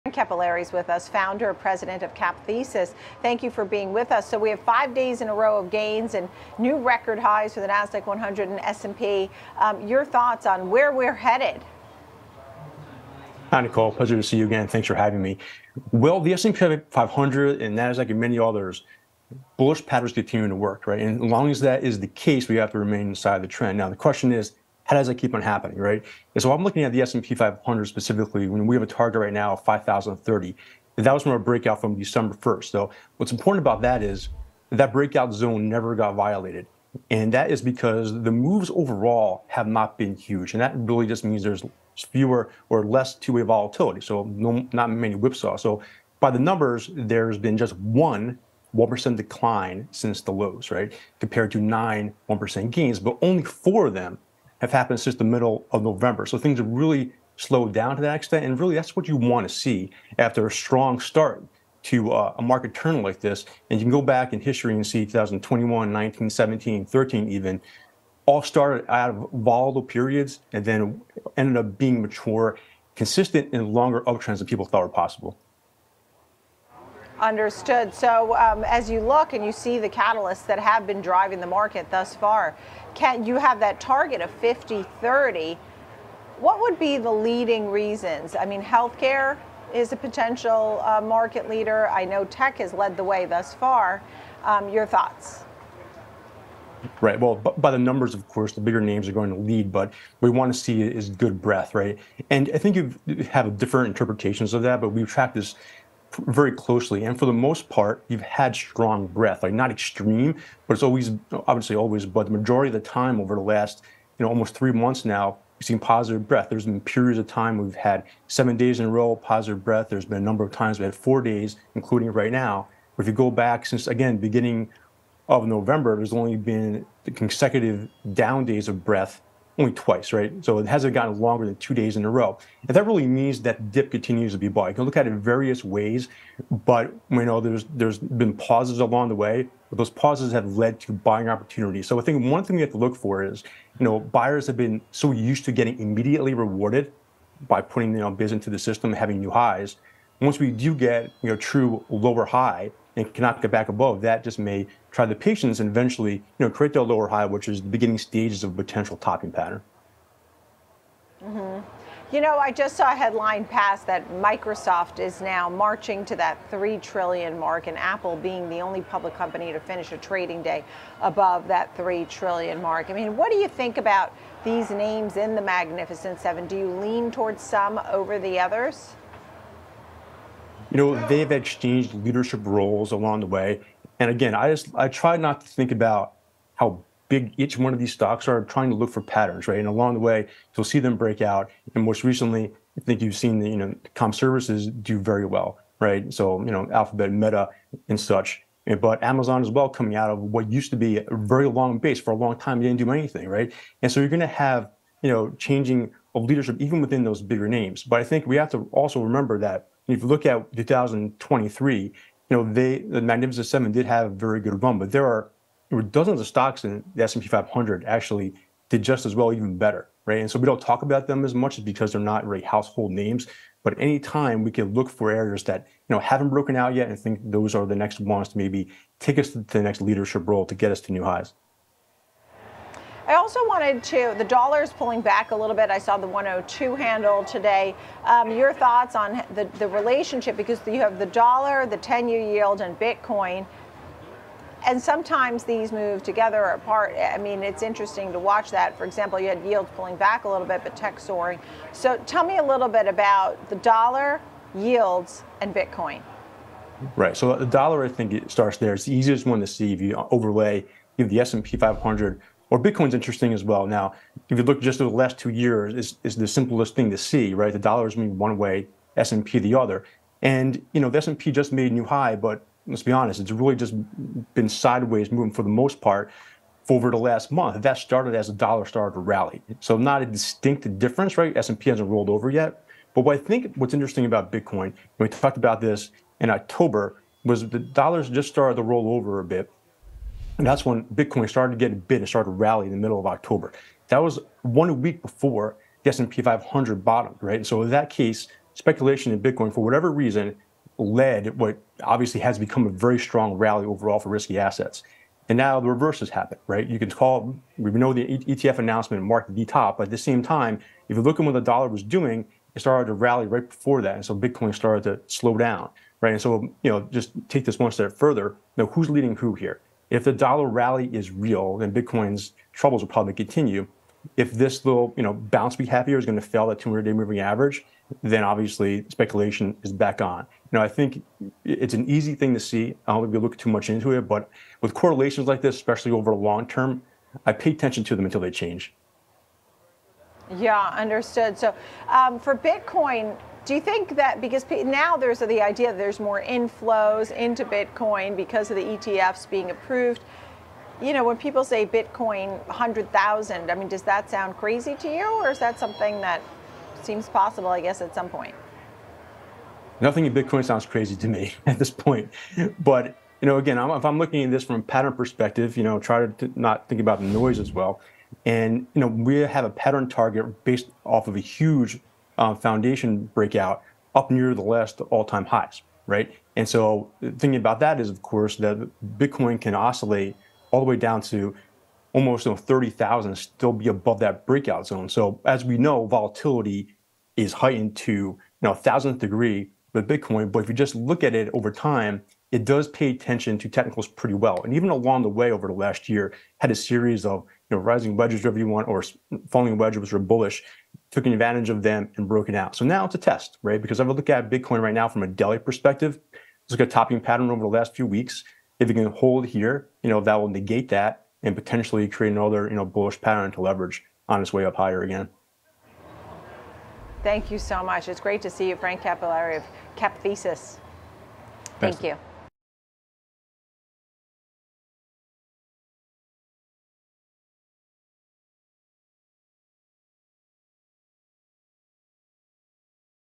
The Negative Correlation Between Bitcoin & The Dollar
Industrials could be a potential next leadership group, notes Frank Cappelleri. He discusses levels to watch on the S&P 500. He evaluates recent equity strength. He then goes over the negative correlation between Bitcoin (/BTC) and the dollar (/DX). Tune in to find out more about the stock market today.